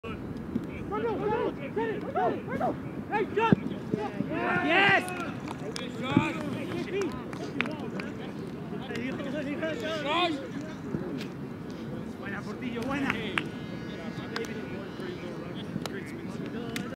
Light Work Against Chicago Fire 👨‍🚒 | New Mexico United
Amando "Sayajin" Moreno and Josh Dolling combine to score our third goal against Chicago Fire!